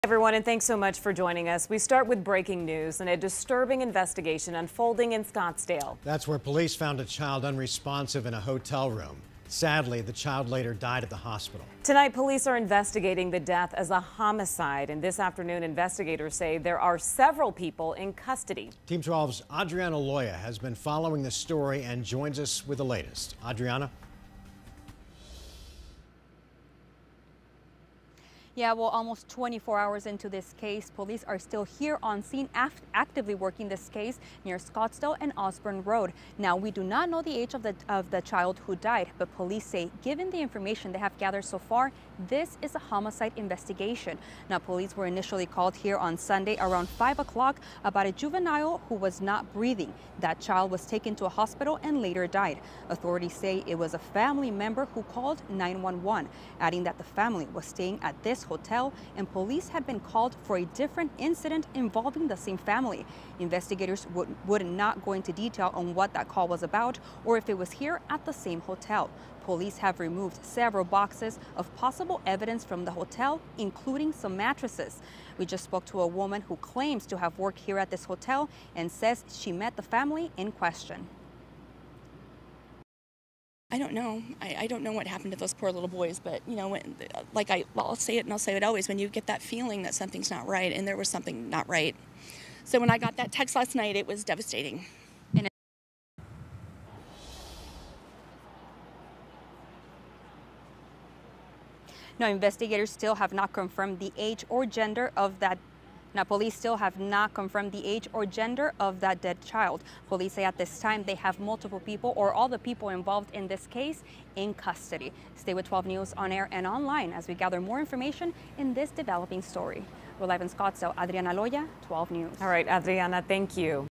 Homicide investigation underway after child found dead at Scottsdale hotel
A child is dead after being found unresponsive in a Scottsdale hotel, officials said. Officials said there are suspects in custody.